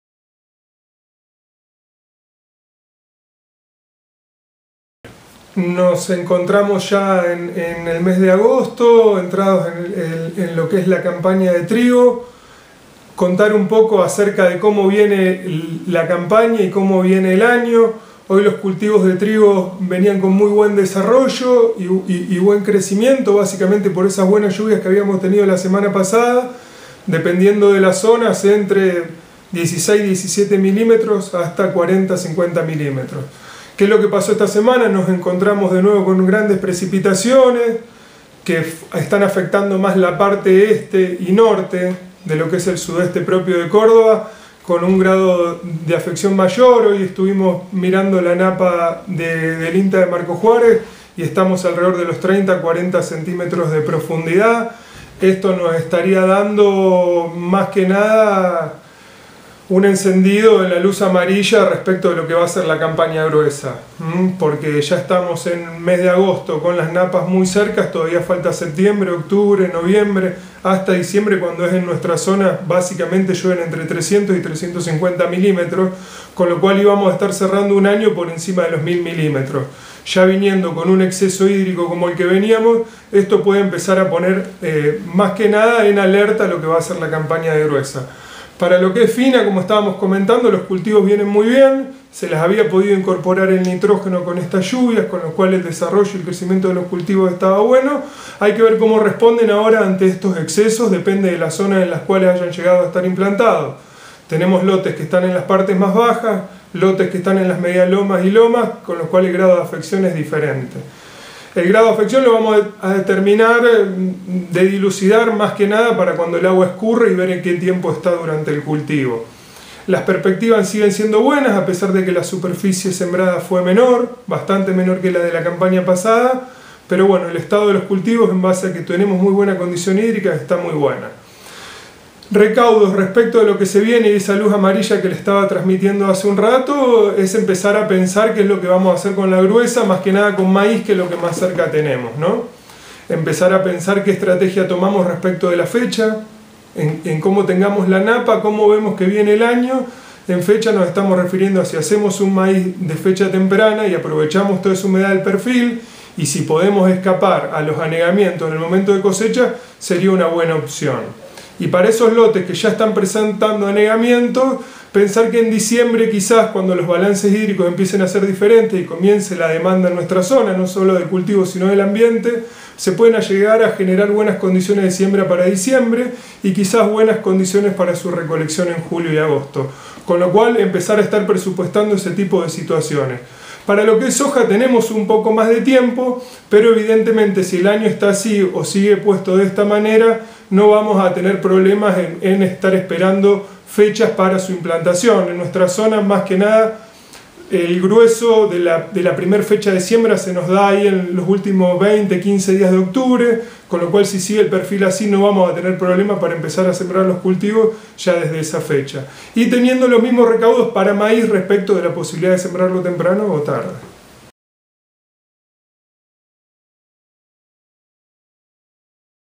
INTA Marcos Juárez - Por el lote - El estado de los cultivos de cosecha fina en la zona
Luego de copiosas lluvias, la perspectiva de lotes sembrados, el estado de los cultivos y el panorama extendido hacia la gruesa. Por Juan Pablo Ioele (INTA AER Corral de Bustos)